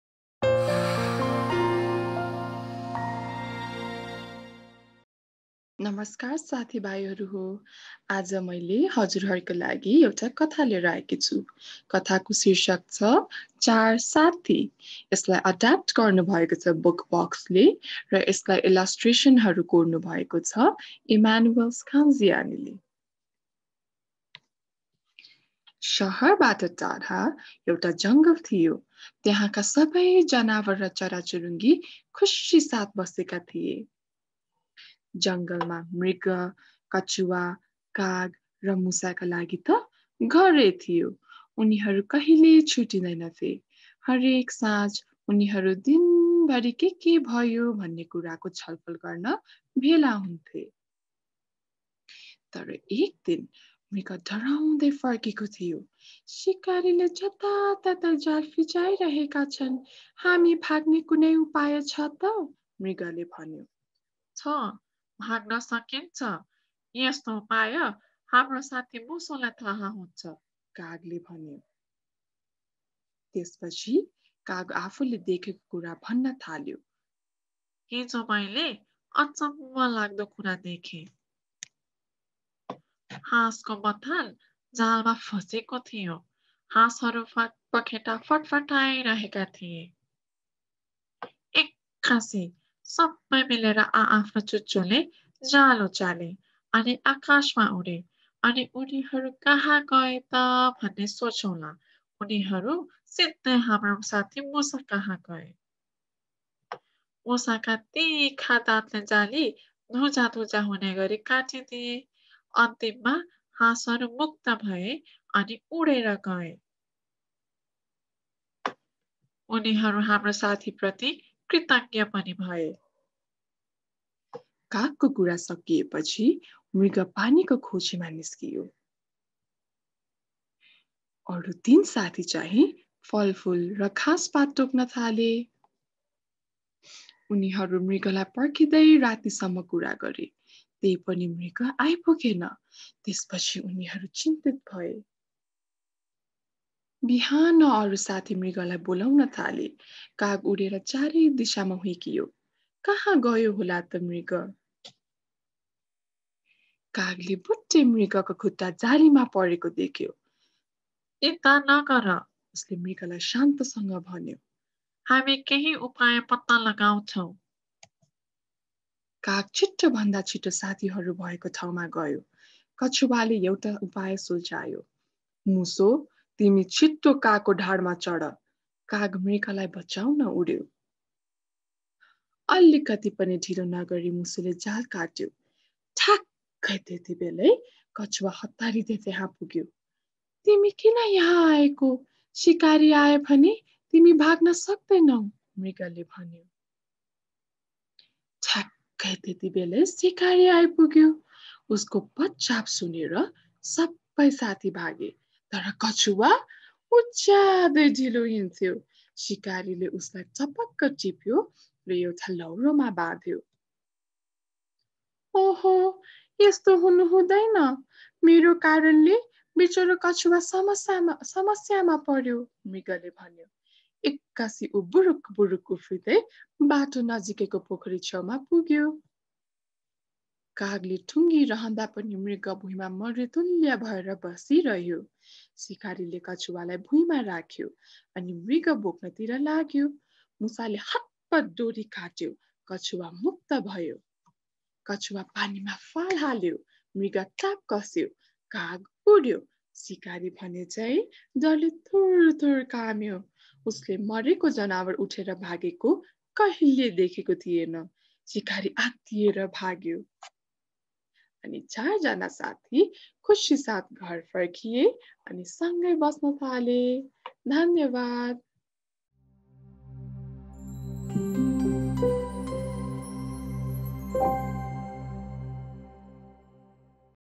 Katha4Nepal || Aakriti Rana || Char Saathi
Katha4nepal is a social initiative to bring learning through stories for kids in Nepal and beyond.

साथी साथी मिल्यो भने जस्तो सुकै कठिन समस्या पनि समाधान गर्न सकिन्छ । यो कथाले त्यसै भन्छ ।

Adapted by Book Box
Illustrator: Emanuele Scanziani
Publisher: Book Box